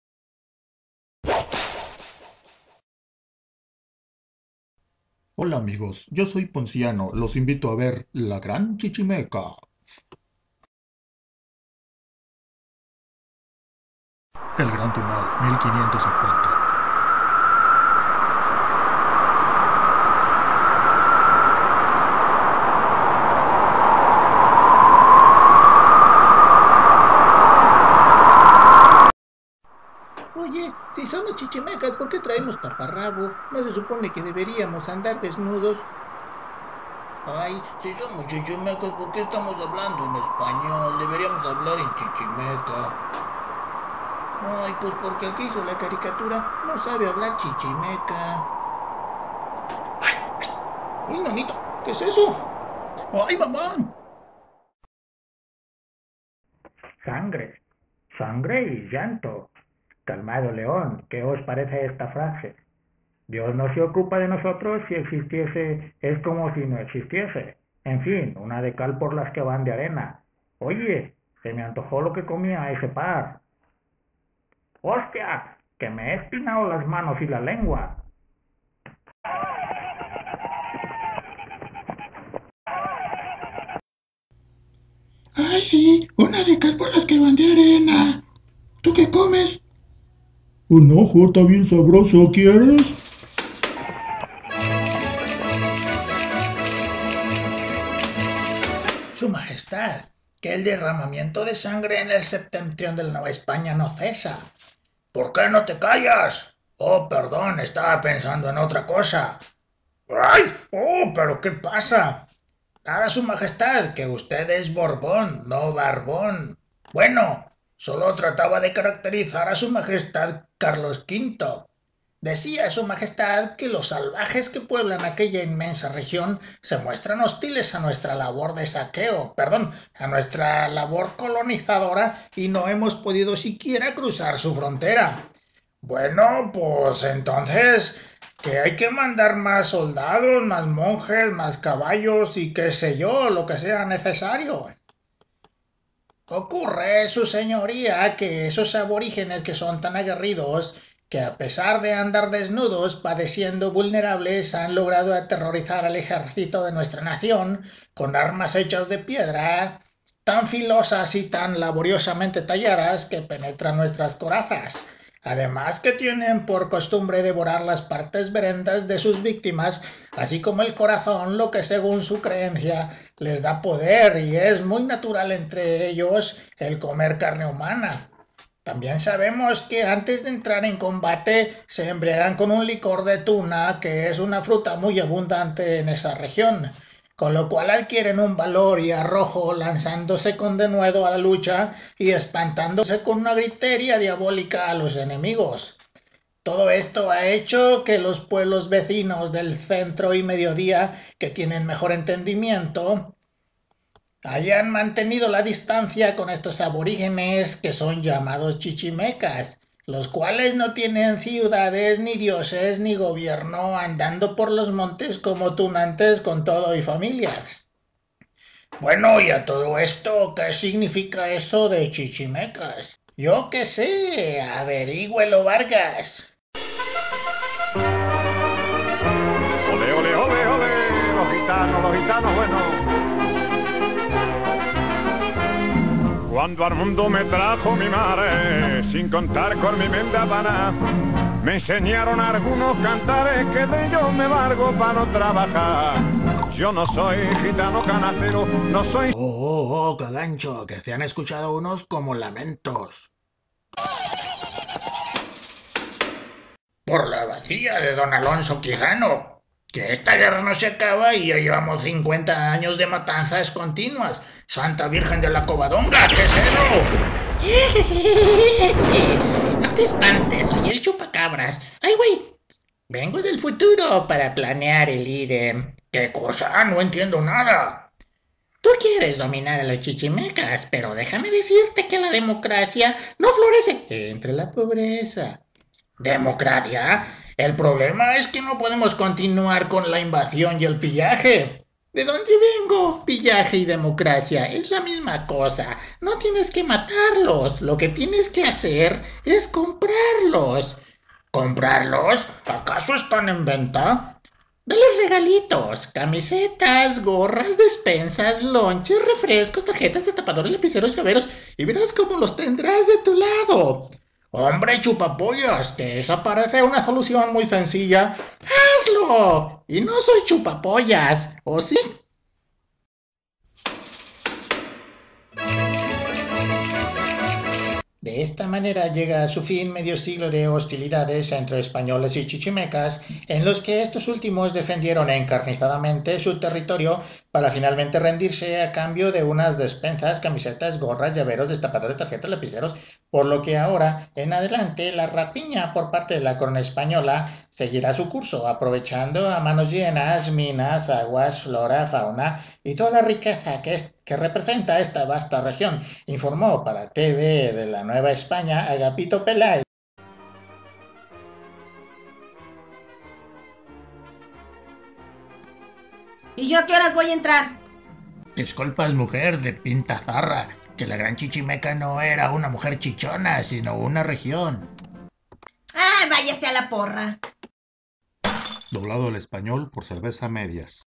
LA GRAN CHICHIMECA
Sátira caricaturesca de la conquista de la Nueva España